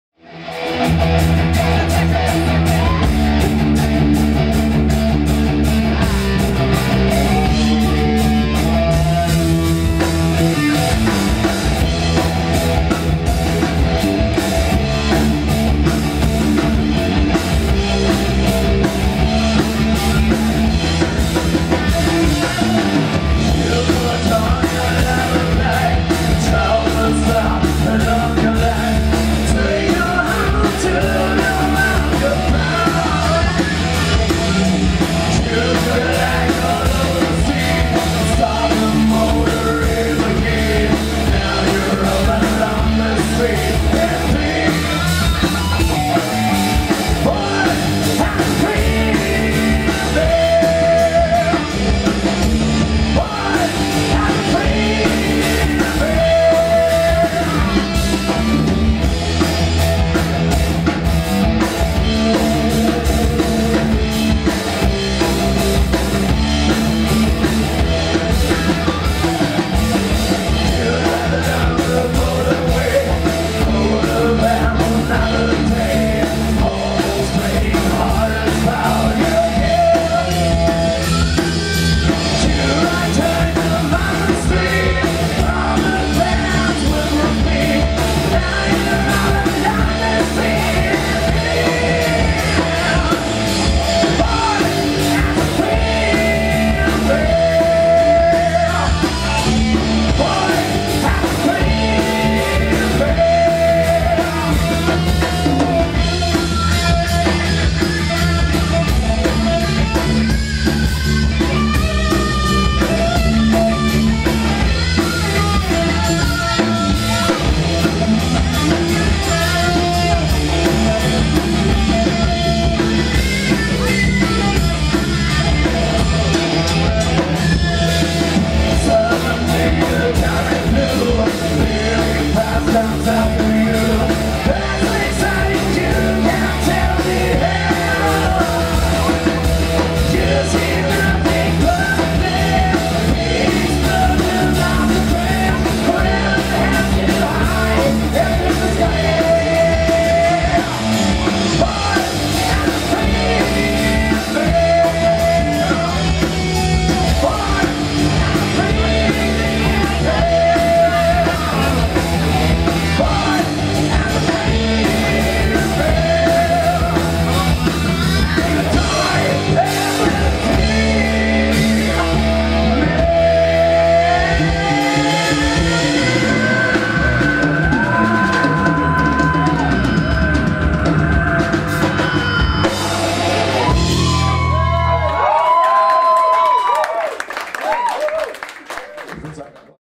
Drachenfelz live @ Schüttorf Rocks 2017
Die Bocholter Band Drachenfelz beim 24. SchüttorfRocks am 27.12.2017 im Komplex in Schüttorf